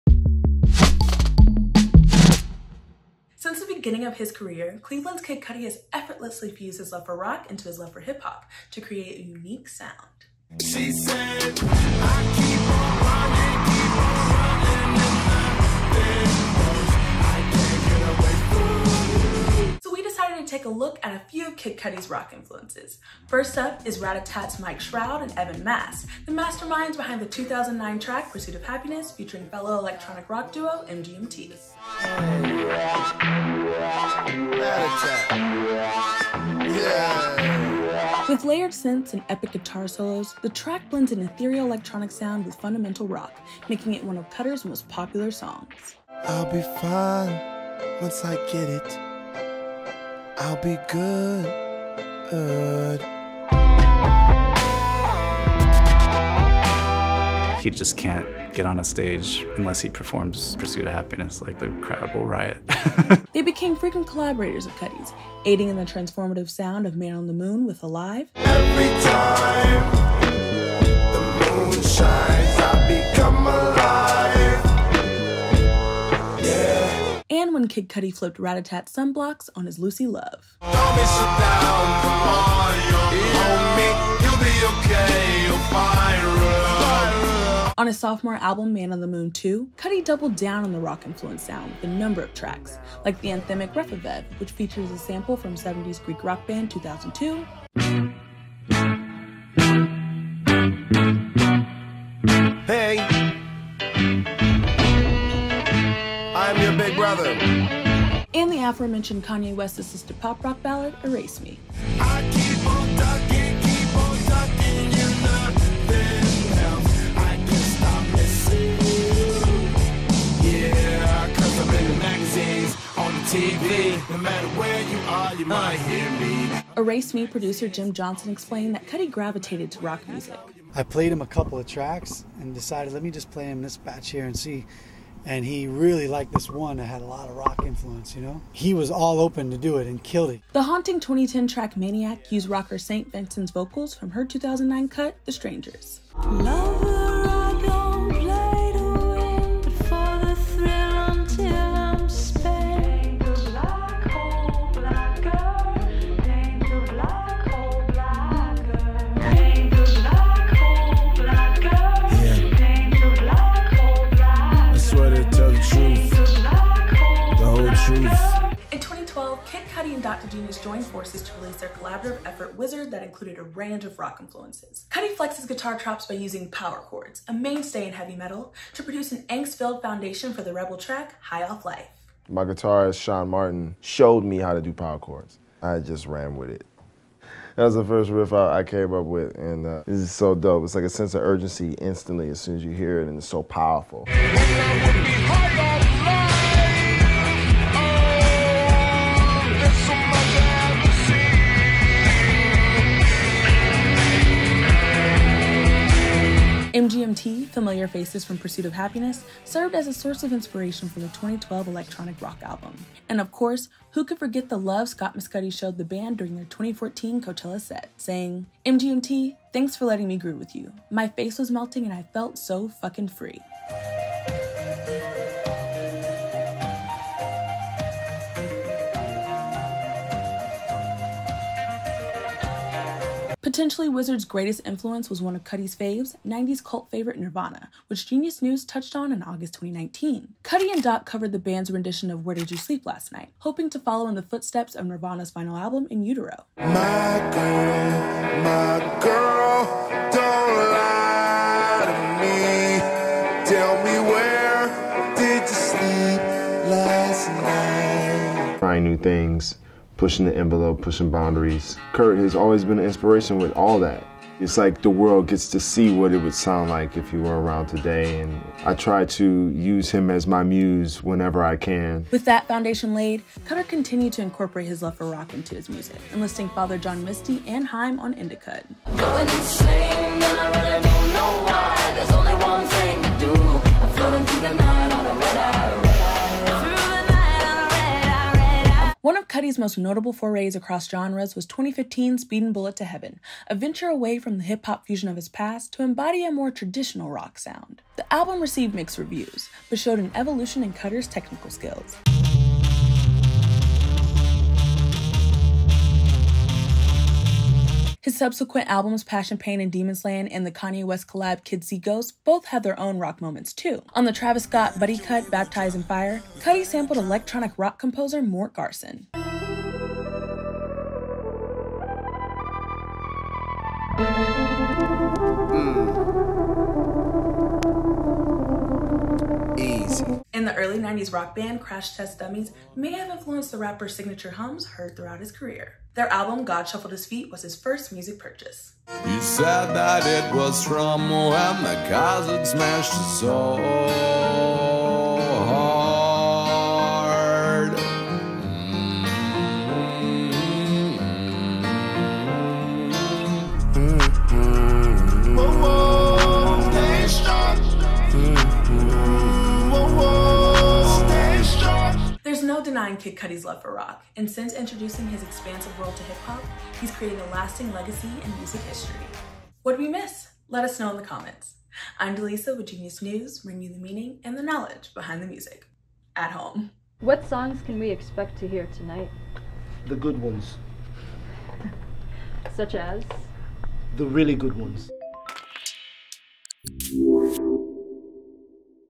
How Kid Cudi Created His Signature Sound | Genius News
Kid Cudi effortlessly fuses his love of rock into his love of hip-hop to create a unique sound, something he’s been doing since the beginning of his career. From collaborators Ratatat, MGMT, Haim, and St. Vincent to his idol Kurt Cobain, guitar players and rock singers are a major influence on the Cleveland rapper’s genre-bending style of music.

CREDITS
Delisa Shannon, Host/Producer/Editor
Jacques Morel, Senior Correspondent
Tia Hill, Supervising Producer